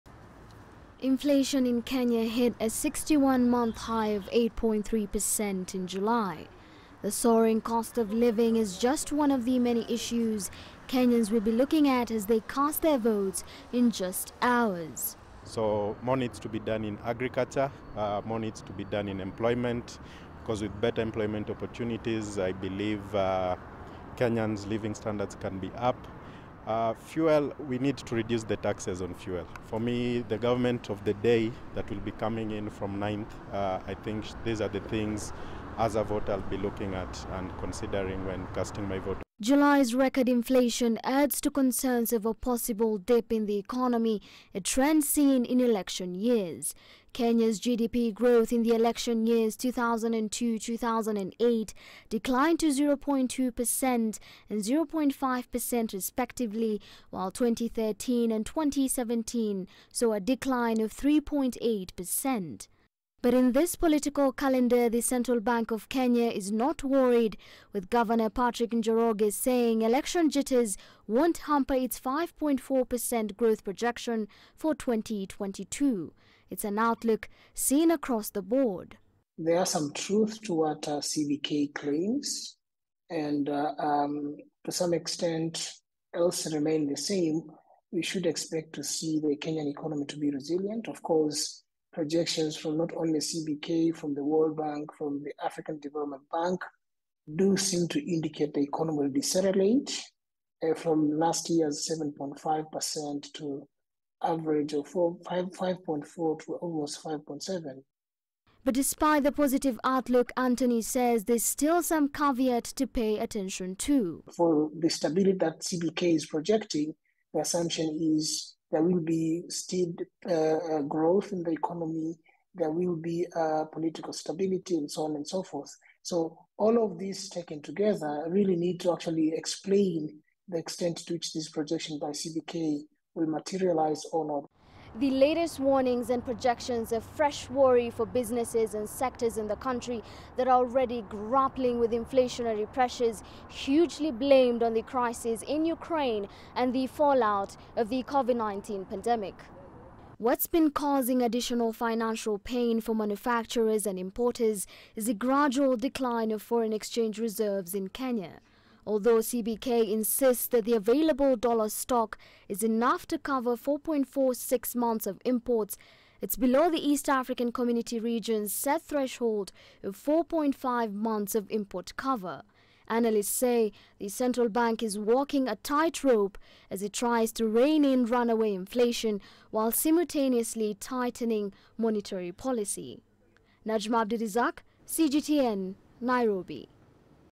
Experts worry economic uncertainty amid elections may haunt Kenya’s growth
Economists see Kenya's economy expanding by at least 5% this year, marking the highest growth in an election year since the return of the multi-party political system more than 30 years ago. But even so, there is uncertainty as record inflation and dwindling foreign exchange reserves put pressure on the economy.